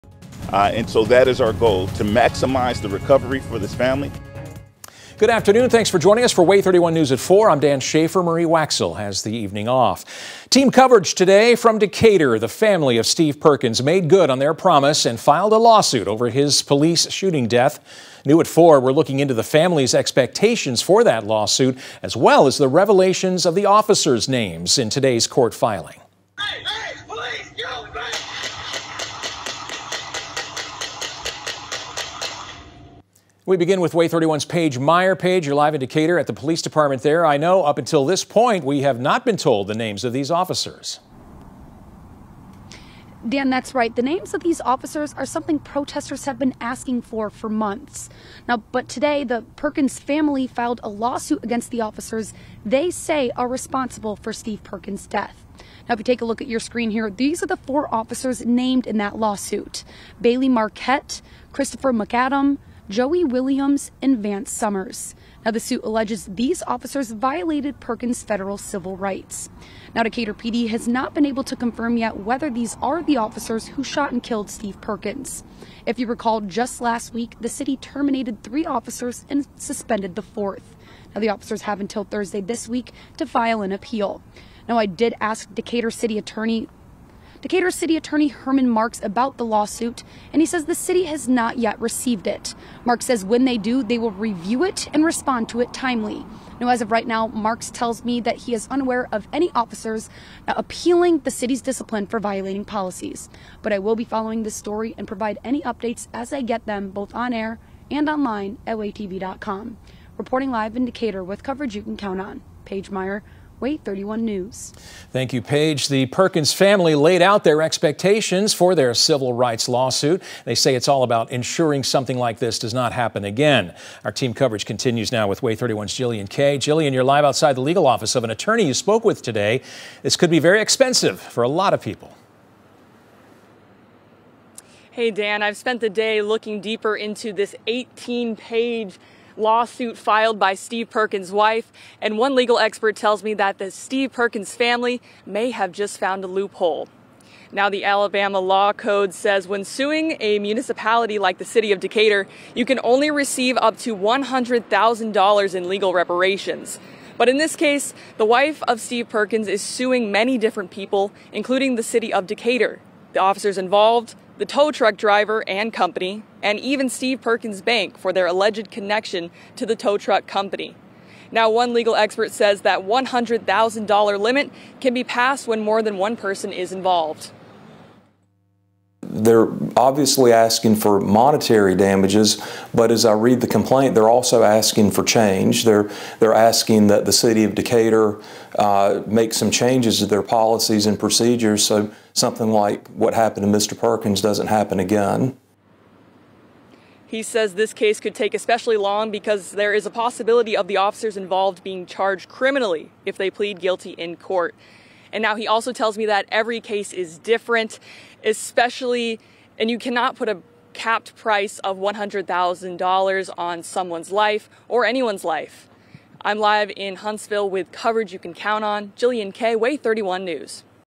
Federal lawsuit names Decatur Police officers linked to killing of Steve Perkins
They are among several being sued.